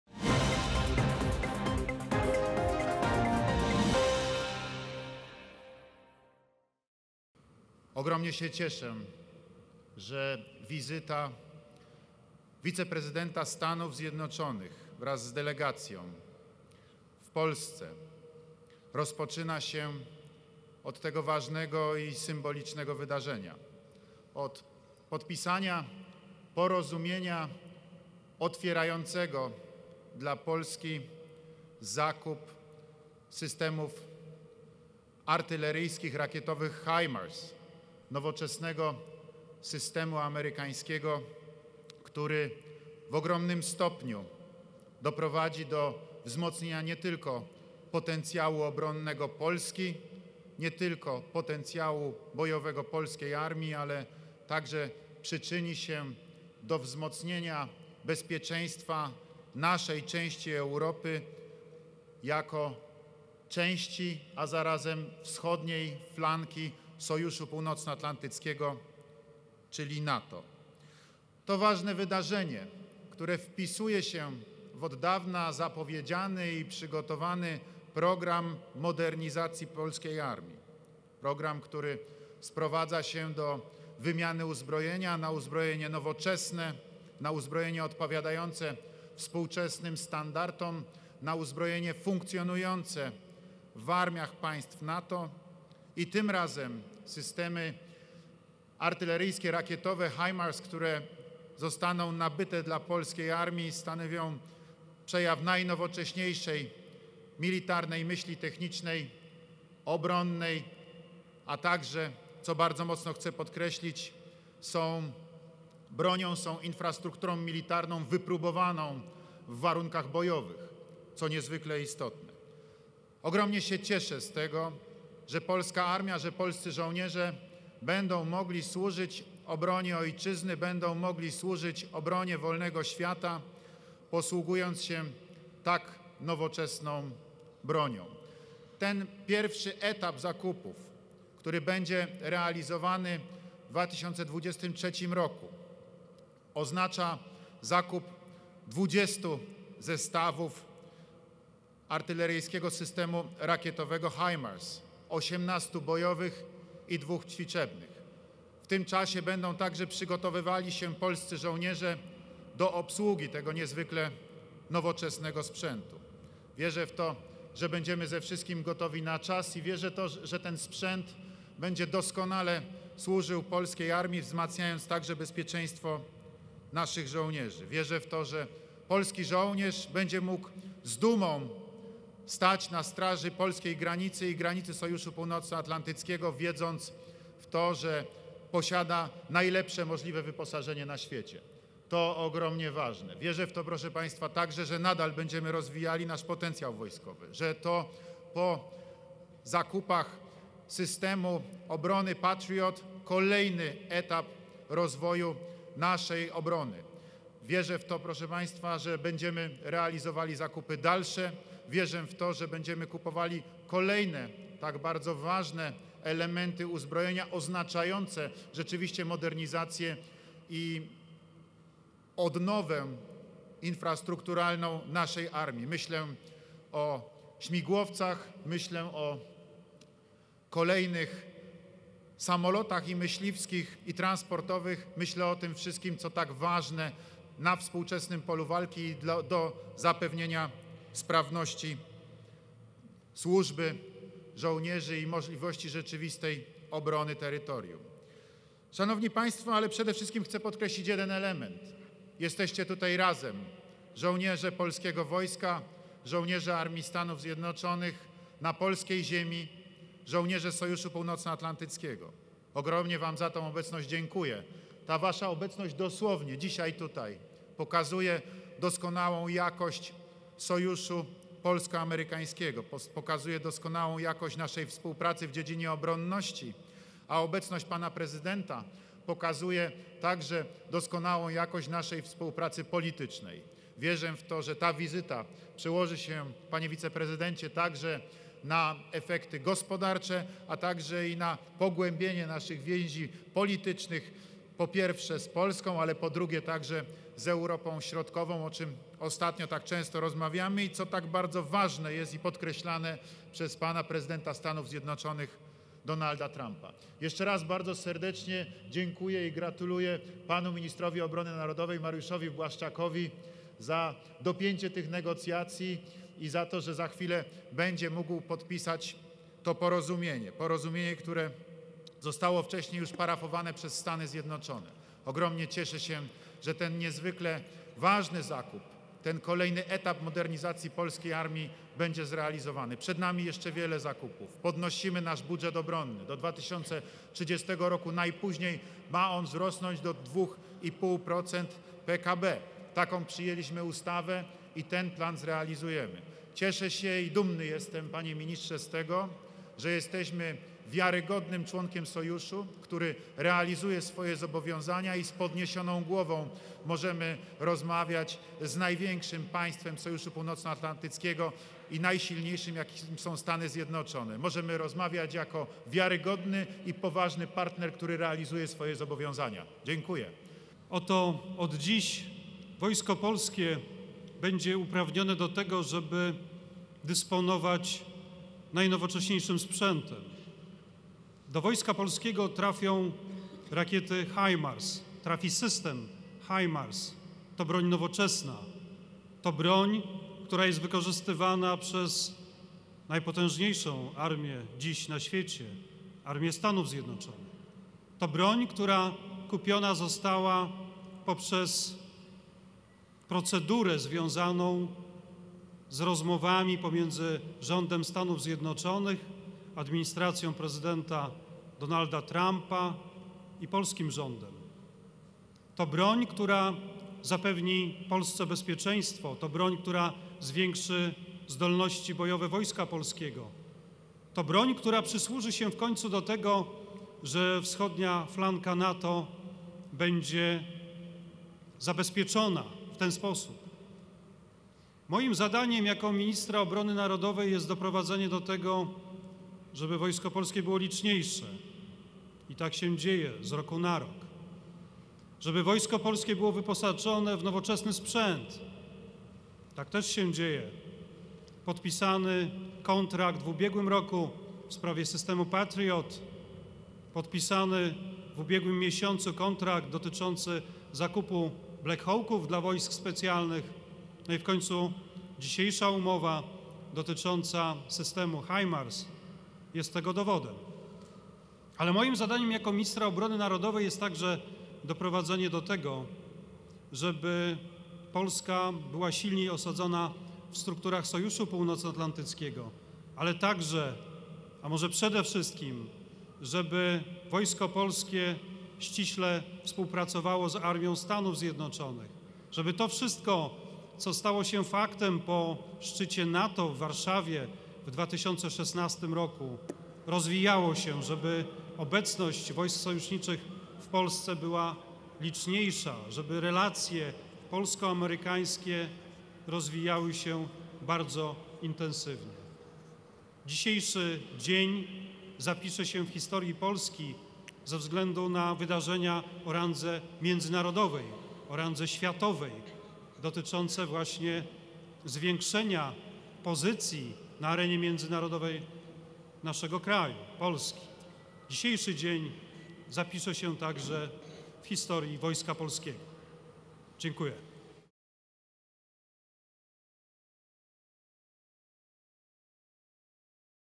"Nowoczesna broń dla polskich żołnierzy", czyli kontrakt na HIMARS-y podpisany
Szef MON Mariusz Błaszczak podpisał w środę umowę z rządem USA na dostawę dywizjonu artylerii rakietowej HIMARS. Zarówno on, jak i prezydent Andrzej Duda podkreślali, że polscy żołnierze będą mogli posługiwać się nowoczesną bronią.